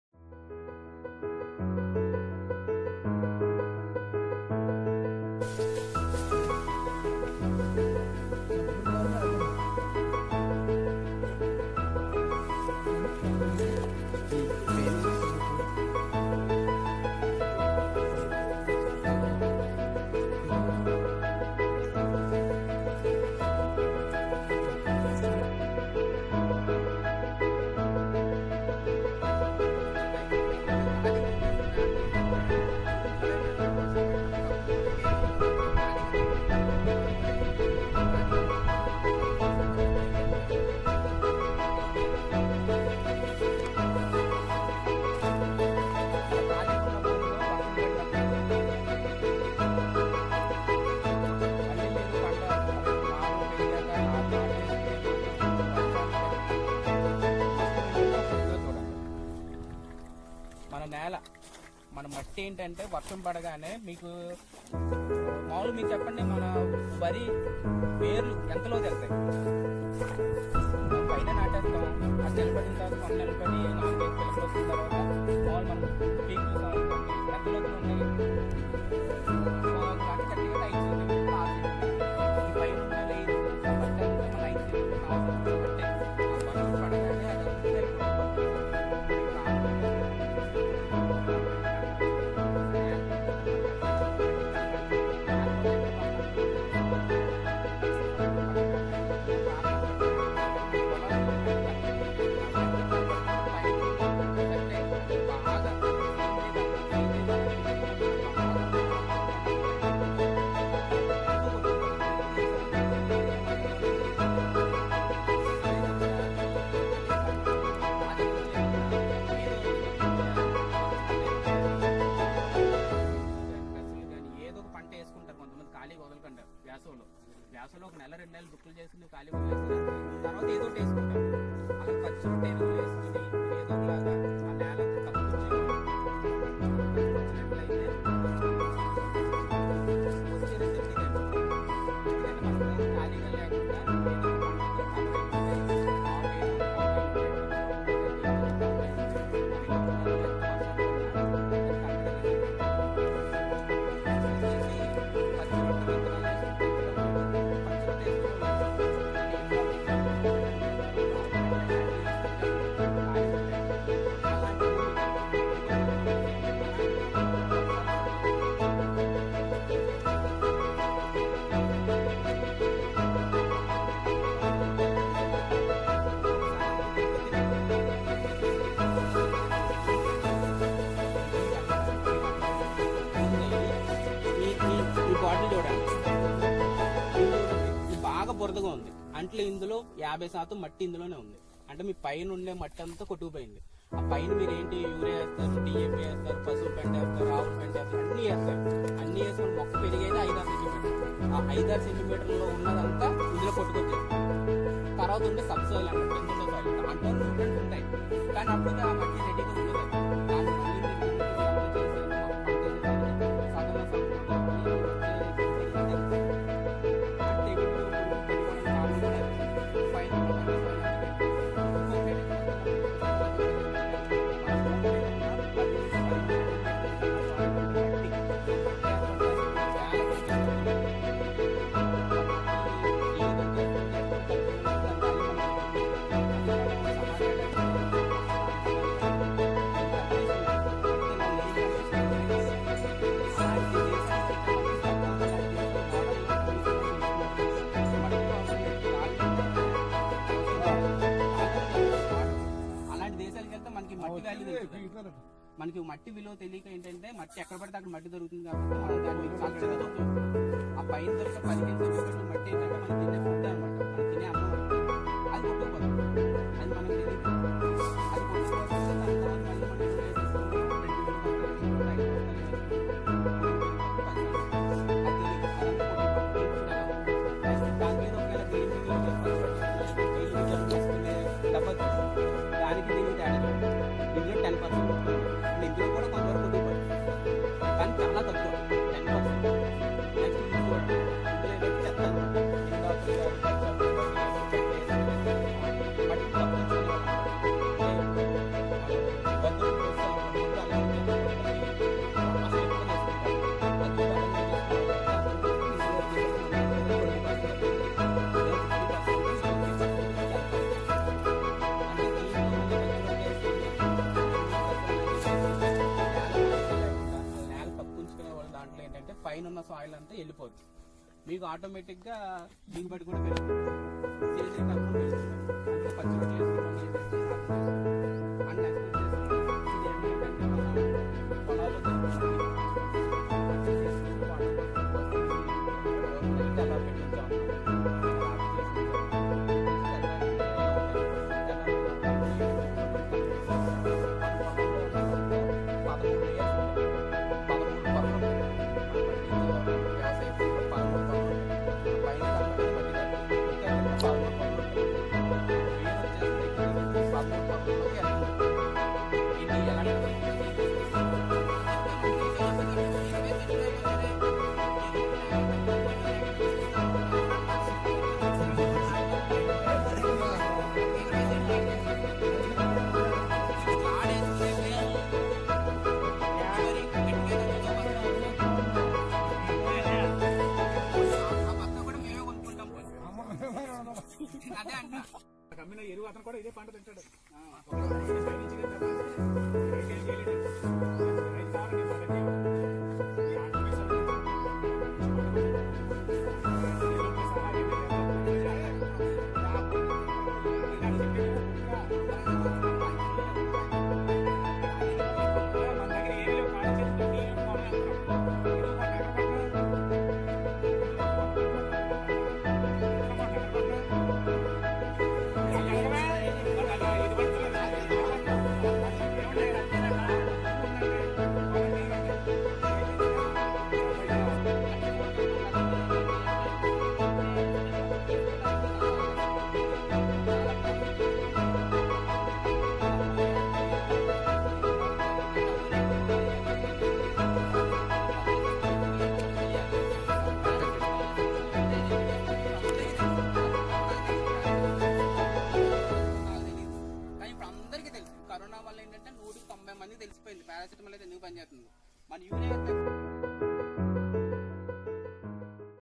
Polambadi Programme 16/3/22
Soil Erosion in Agriculture fields and Soil profile experiments,Nitrogen cycle chart explanation done in this programme